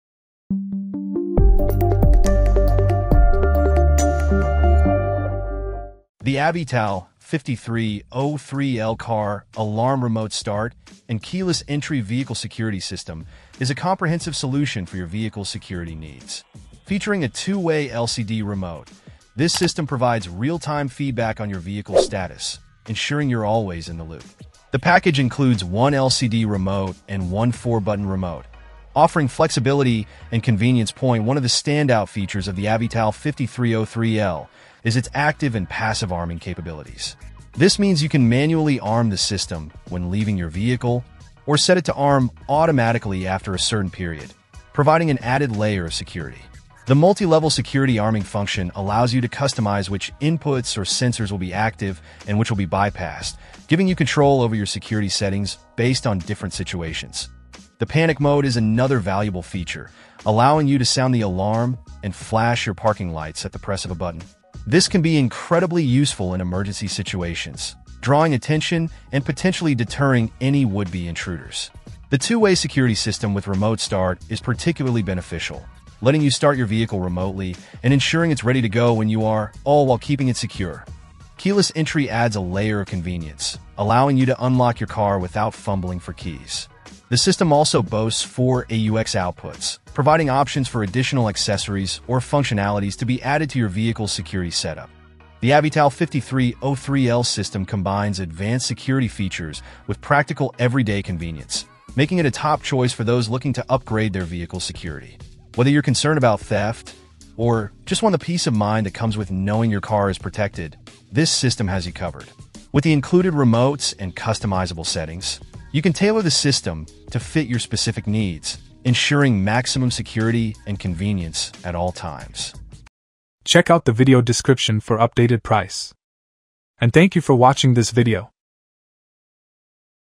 Avital 5303L: The Ultimate 2-Way LCD Remote Car Alarm and Security System
0:00 Intro
0:06 Review

Things that we mentioned in this video:
Avital 5303L Car Alarm : B001JT5KIS